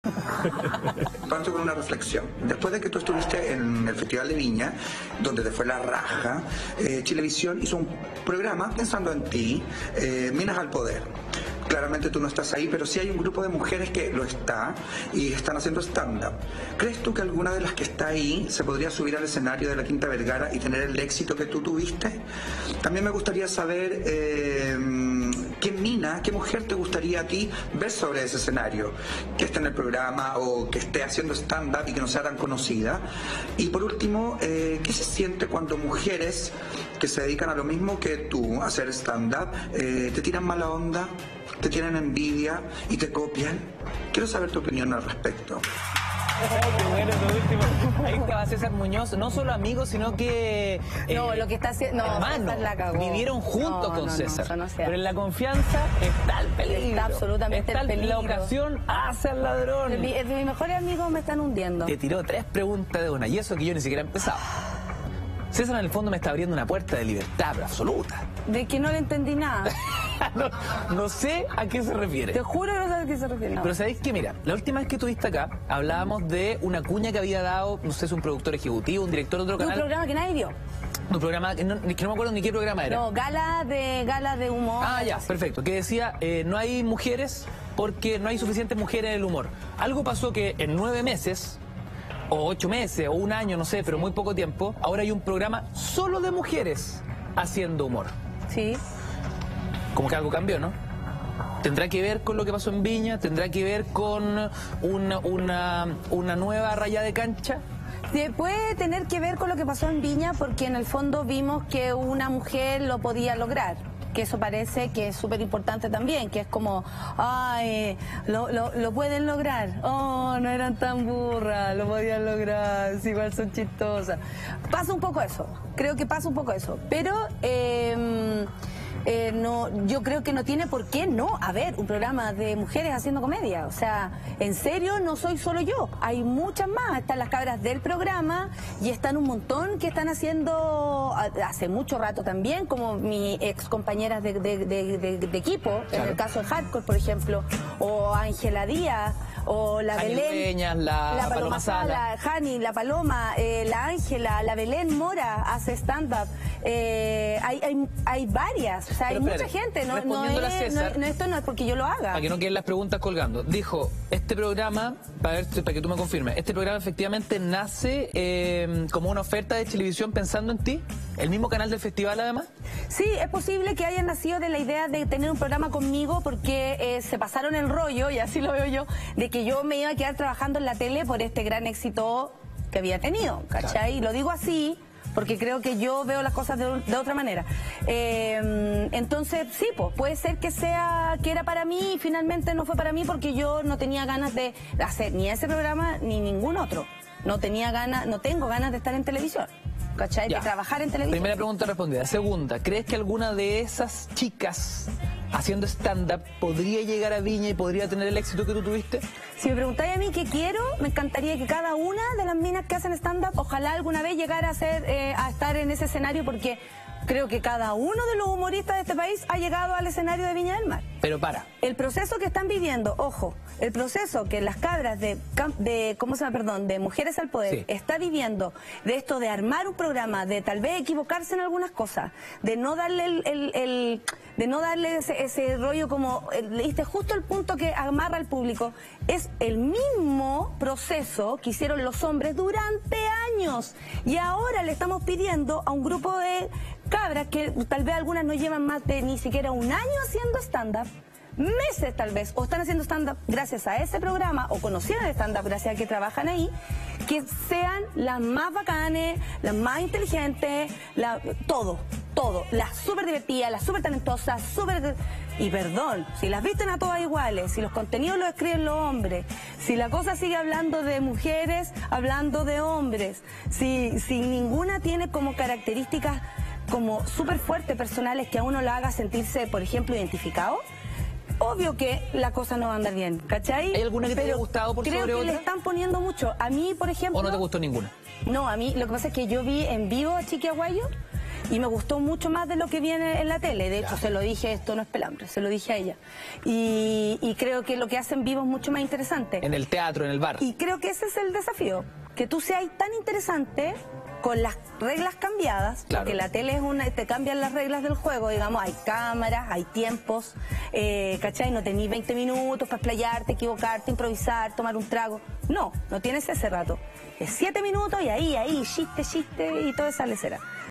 Natalia Valdebenito fue clara: "No quiero volver a trabajar en televisión"
La humorista nacional nos visitó en los estudios de La Red para hablar de su éxito en Viña y también si le gustaría volver o no a la TV.